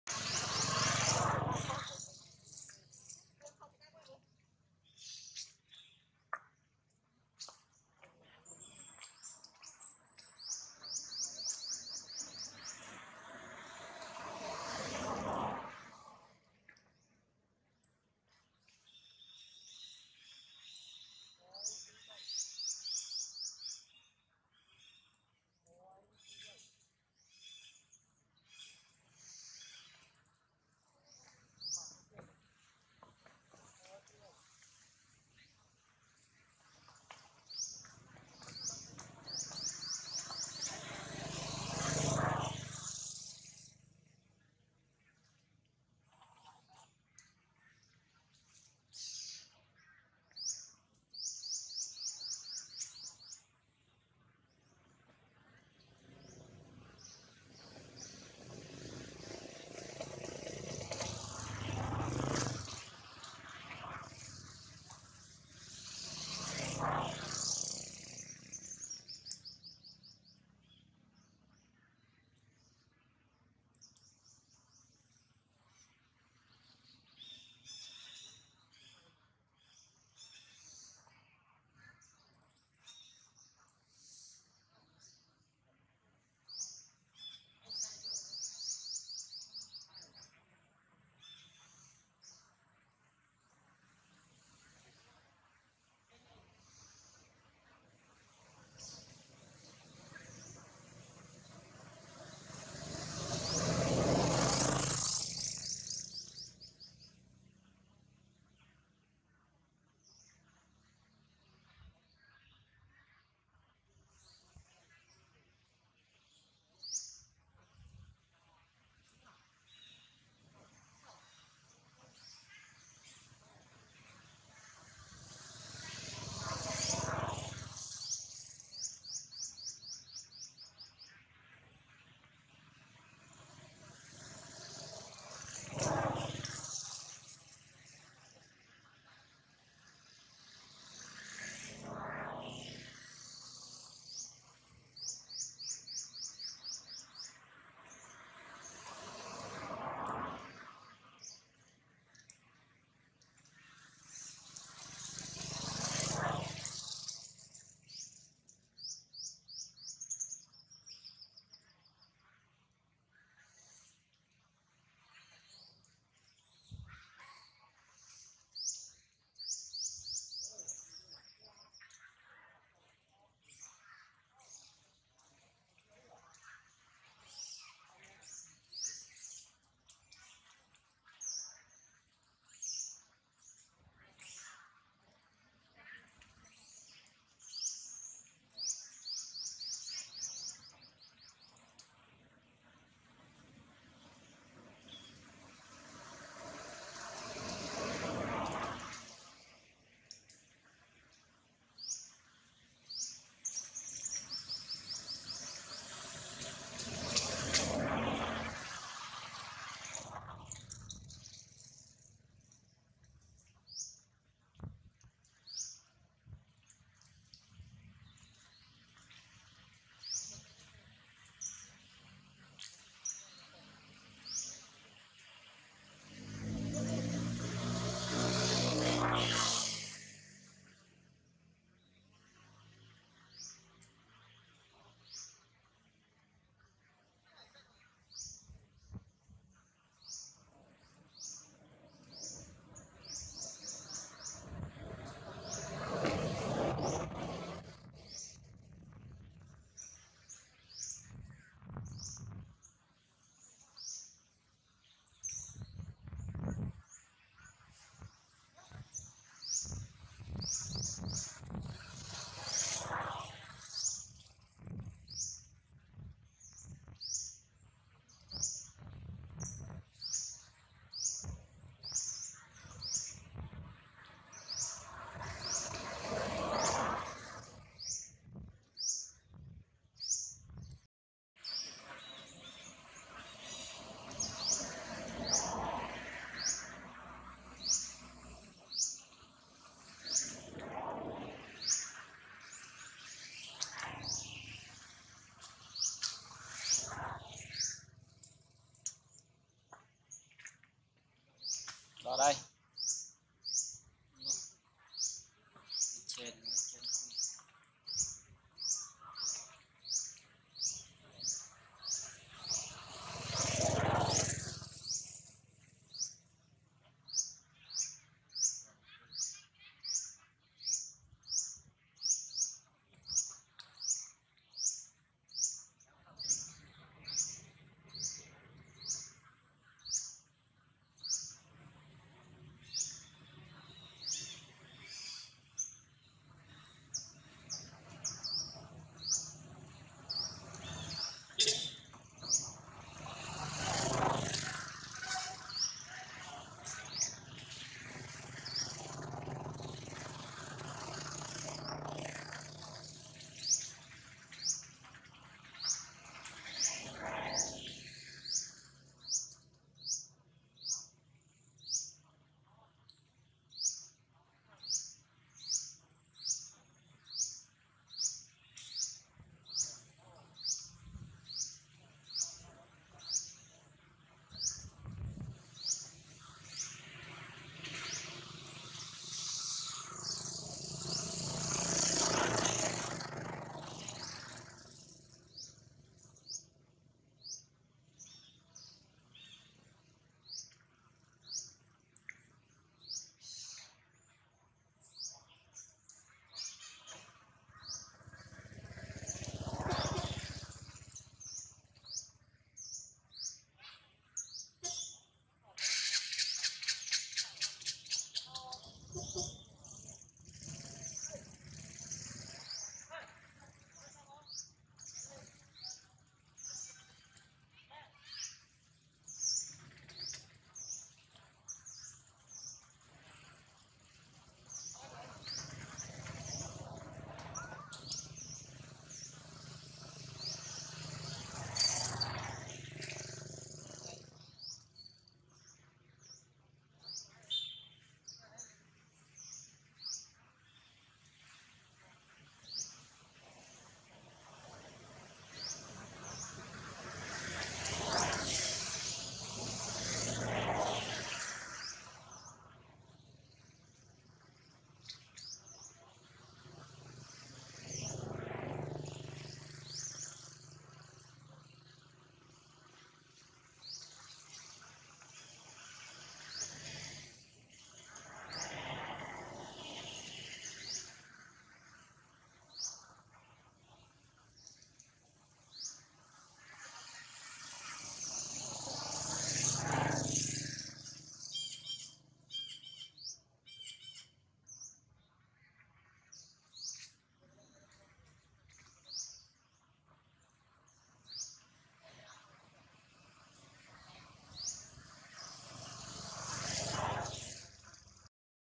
Hurt full ! Stress full ! Pregnant old mom Jane scream loud cuz Jane want to born her new baby soon
Everyday Monkey